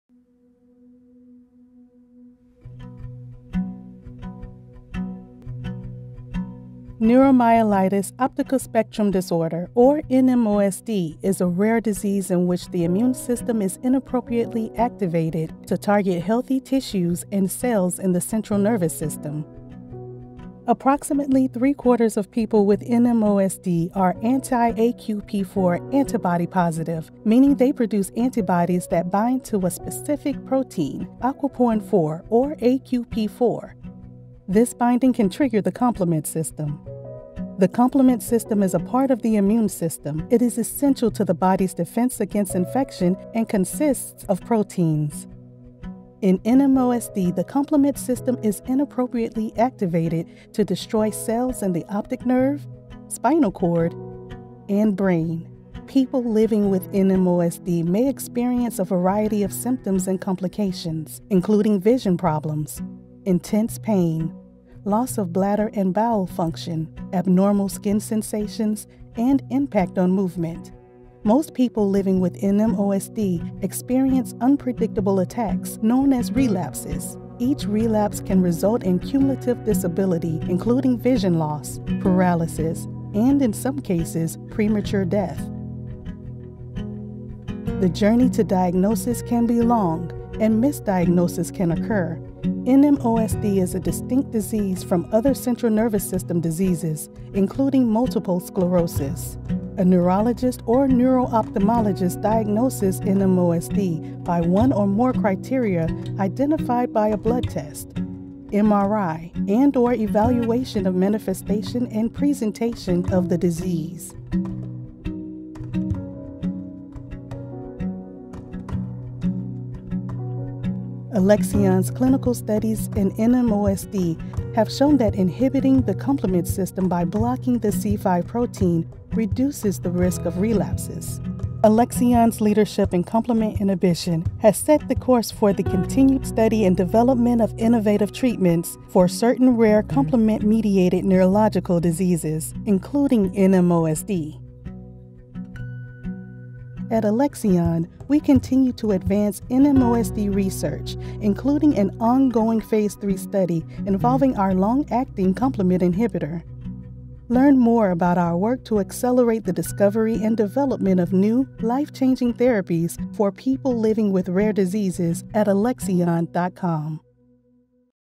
What is Neuromyelitis Optica Spectrum Disorder (NMOSD)?
Neuromyelitis Optica Spectrum Disorder (NMOSD) is a rare, autoimmune disorder of the central nervous system that affects the optic nerve and spinal cord, more commonly in women. The disorder is characterized by unpredictable attacks, known as relapses, that result in cumulative disability, including vision loss, paralysis, and sometimes premature death.